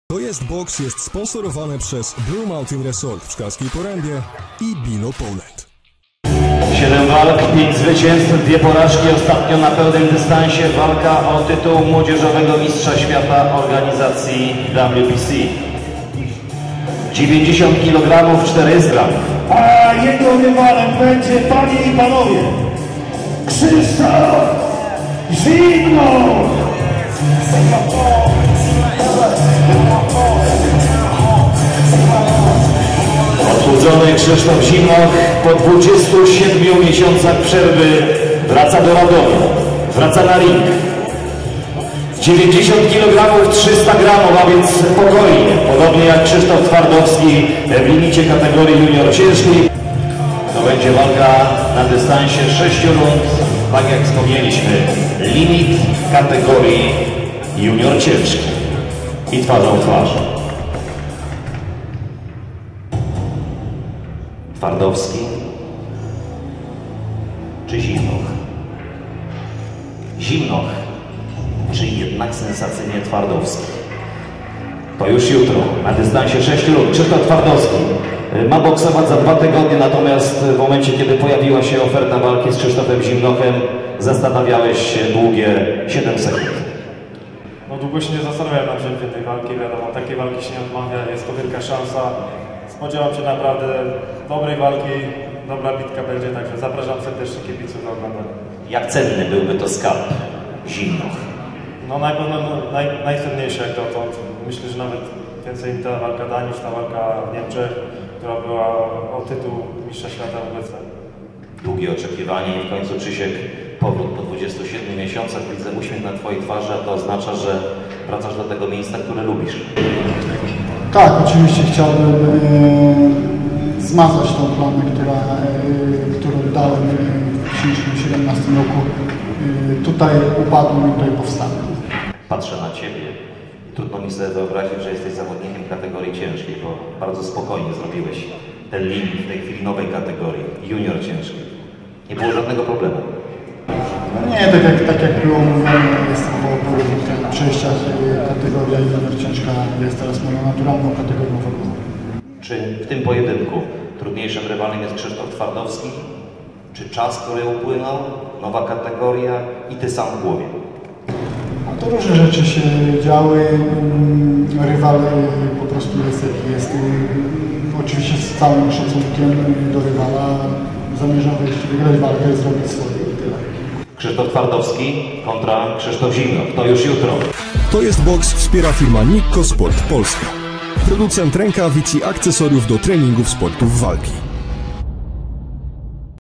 Krzysztof Zimnoch po raz pierwszy w junior ciężkiej! - ważenie MB Boxing Night
- zapraszamy na naszą stronę. Codziennie nowe informacje o boksie